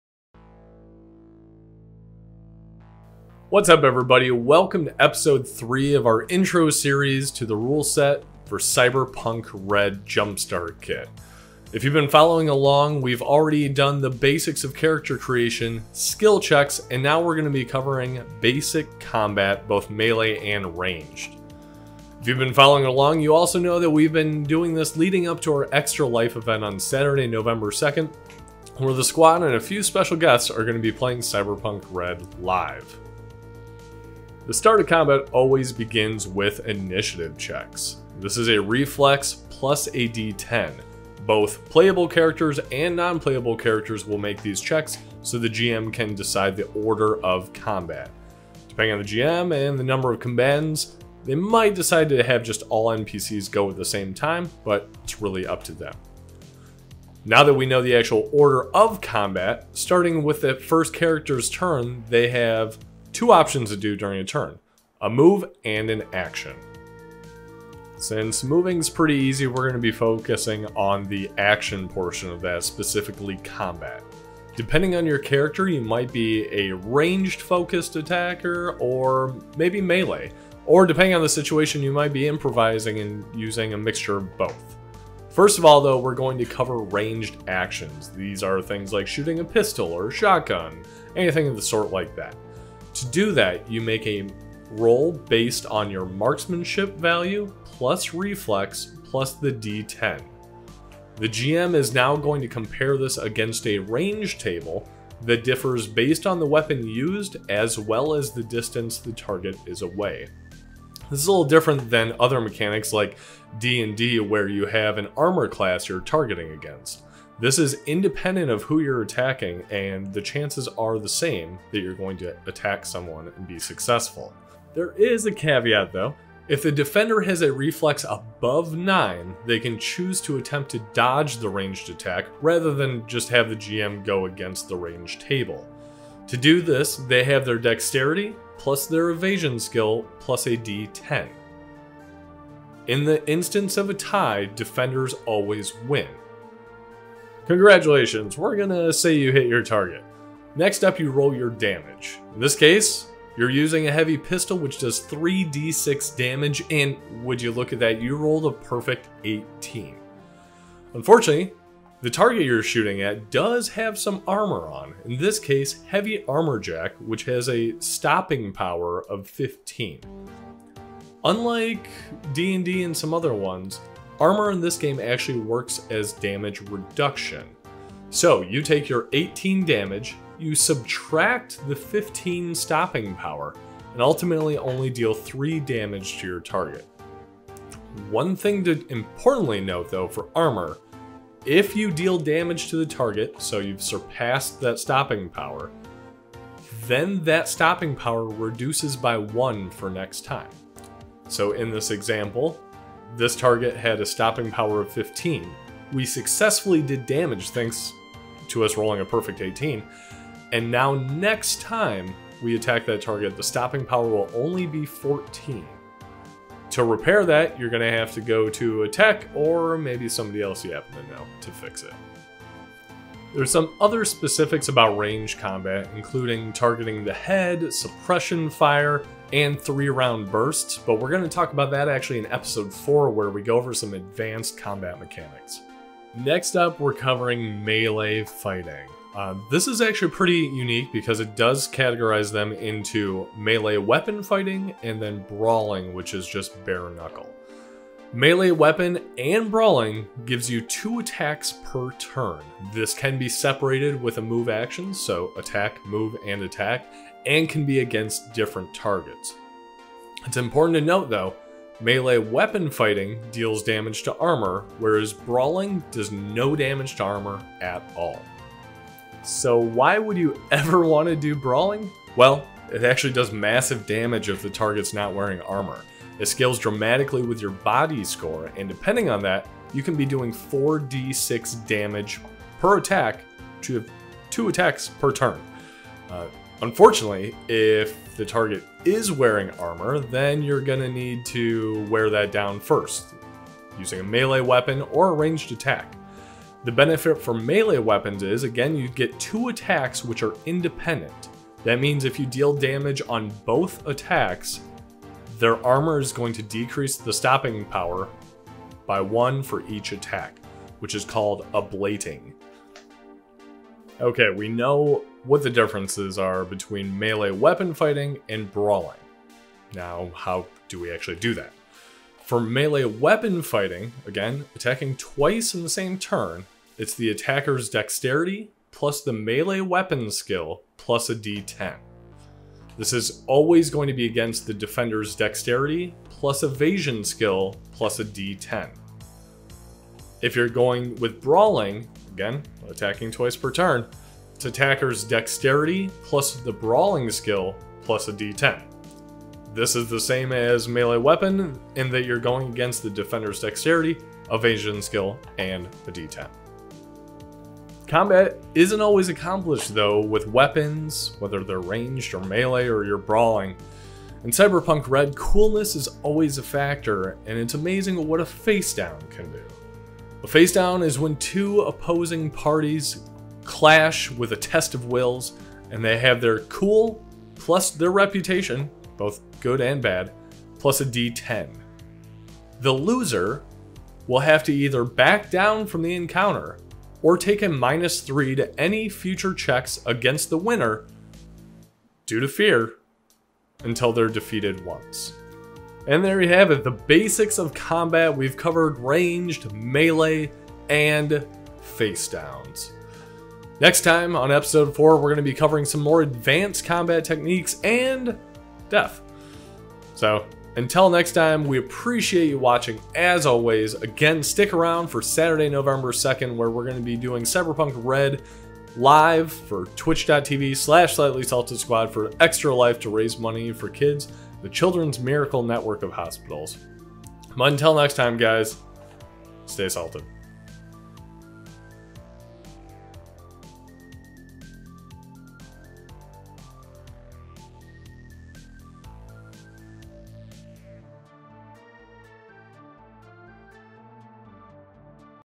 Cyberpunk Red Jumpstart Kit: Basic Combat
Welcome to a limited series focused on the tabletop RPG: Cyberpunk Red. Throughout several episodes I'll detail out the mechanics of Cyberpunk Red's Jumpstart Kit!

Check us out for a live session on November 2nd for our ExtraLife stream at Twitch.tv/SlightlySaltedSquad